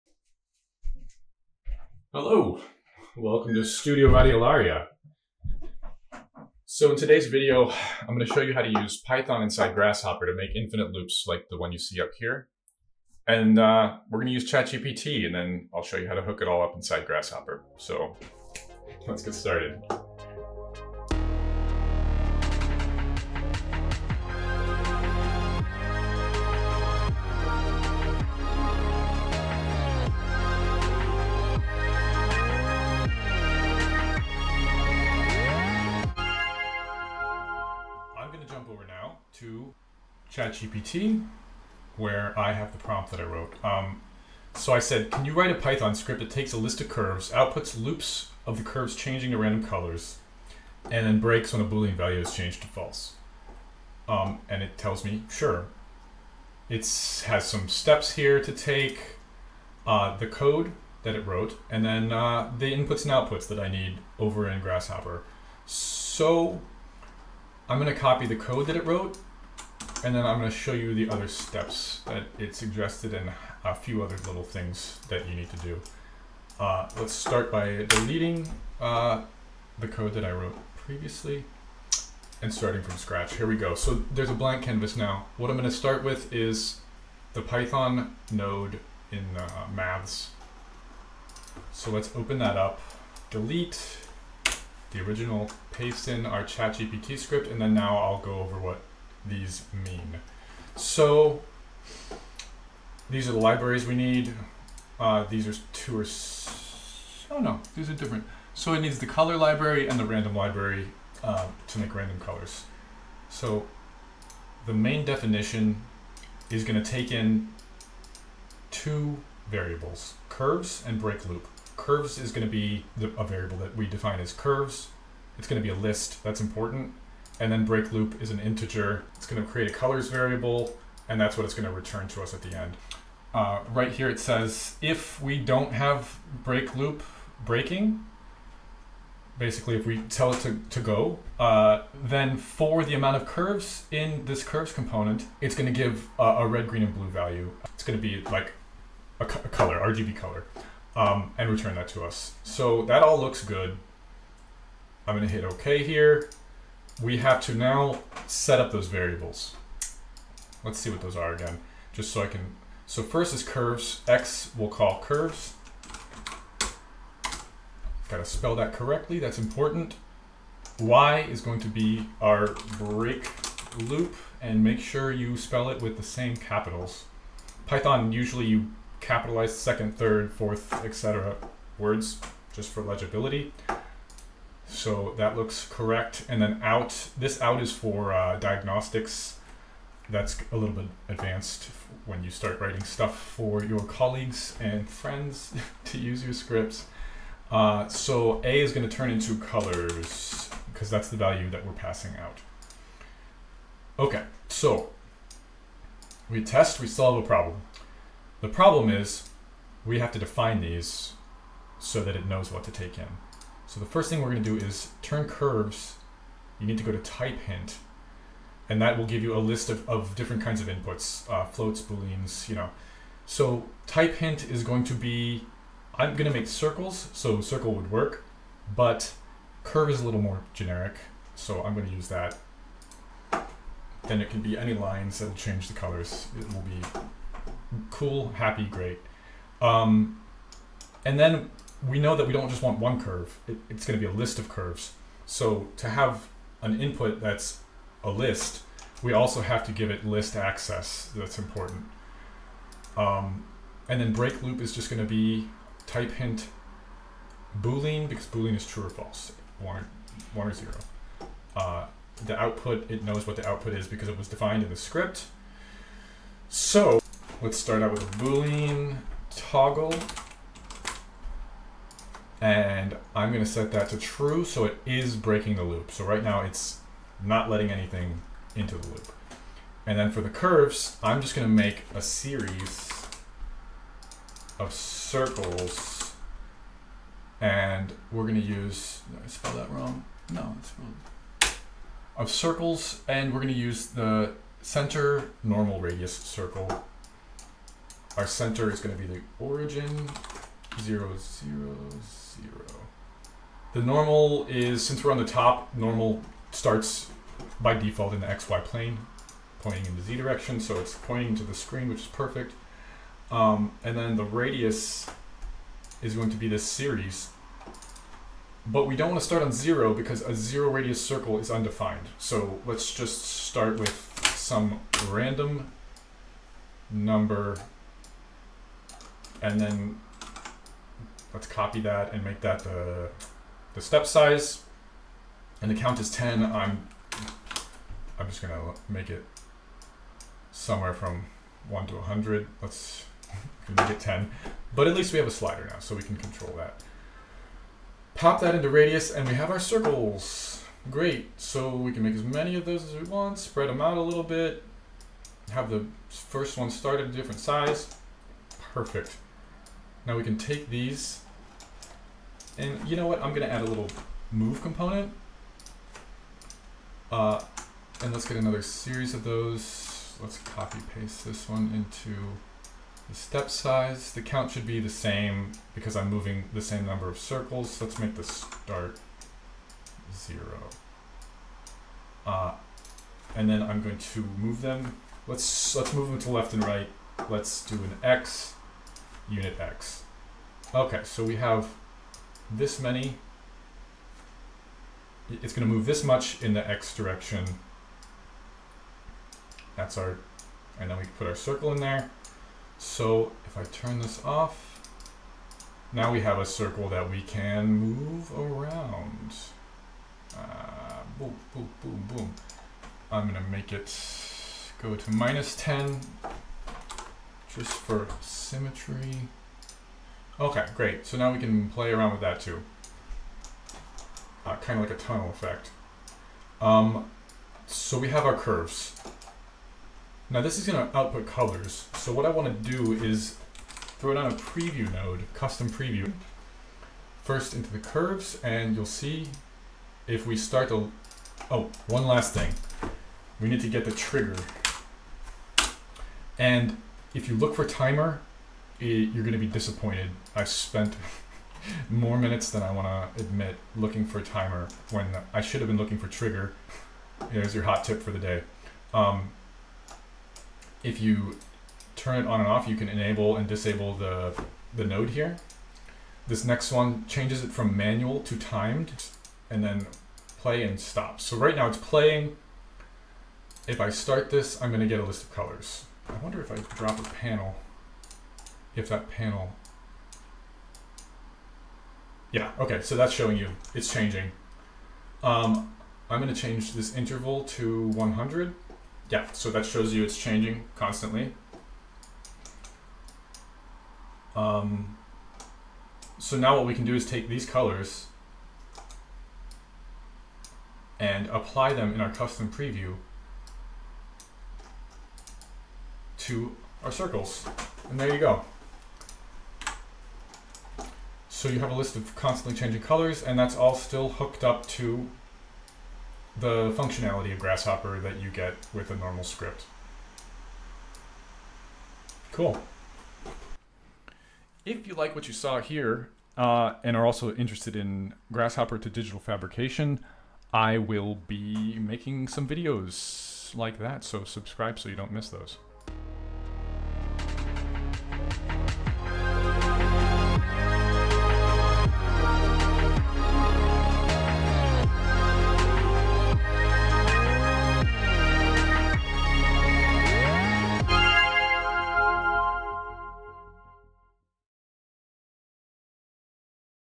Use python code in grasshopper. Generate ChatGPT AI Python Code and run in Grasshopper
This tutorial will walk you through promting chatGPT and setting up coding ideas as a python node inside grasshopper.

0:00 Description
0:20 Intro
0:37 ChatGPT Promt
1:26 Python Node
2:36 Variable Setup
4:27 Create Grasshopper Script
8:44 Outro